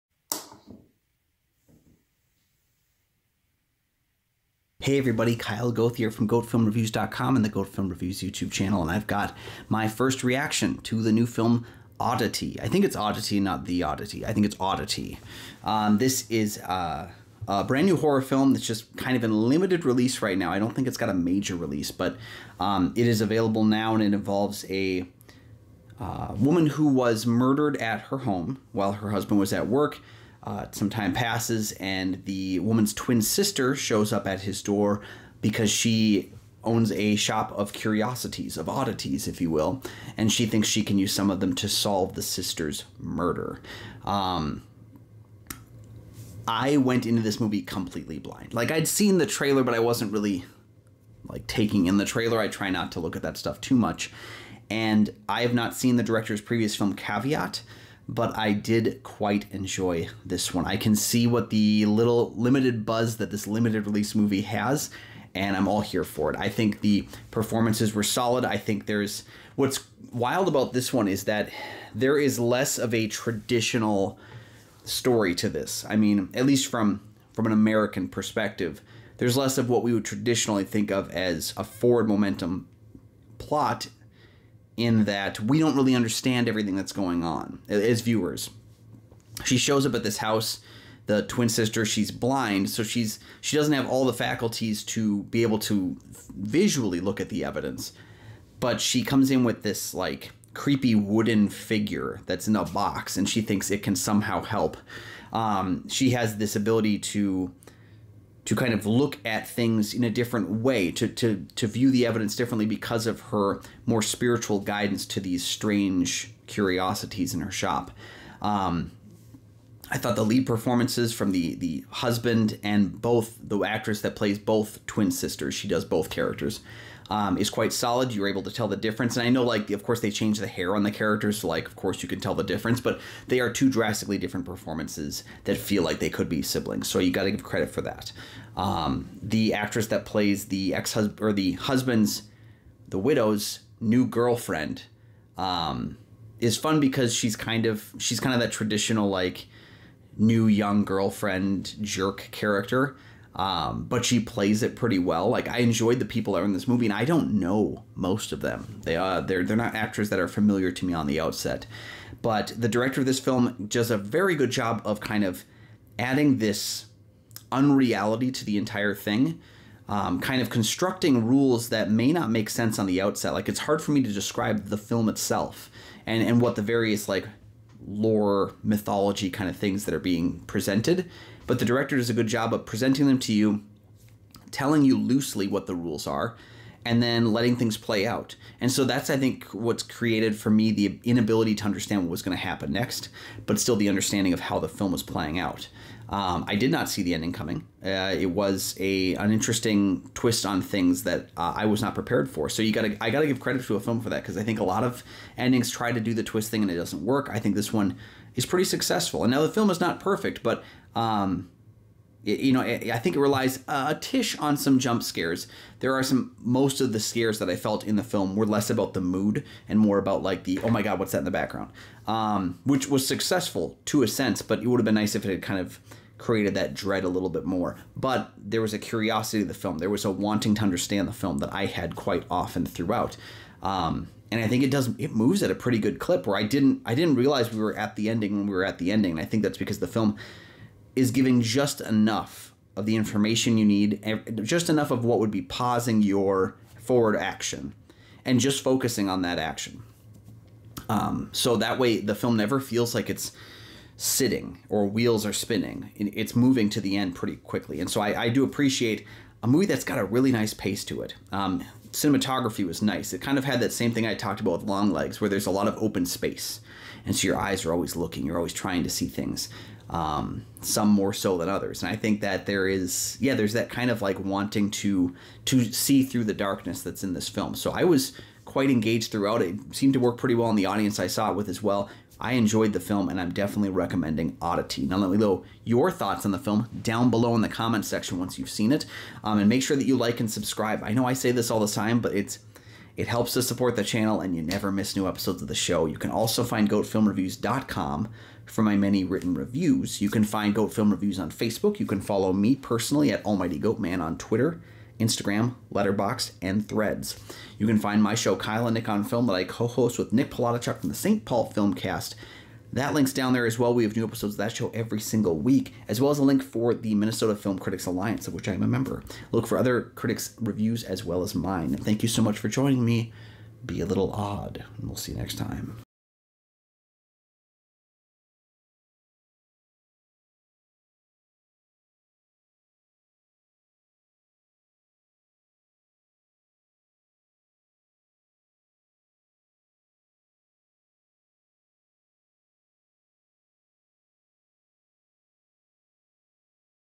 Oddity (2024) - FIRST REACTION
First Reactions immediately following my first viewing of Damian Mc Carthy's Oddity (2024). These thoughts are not rehearsed, and they may even change once I've had time to ruminate on them.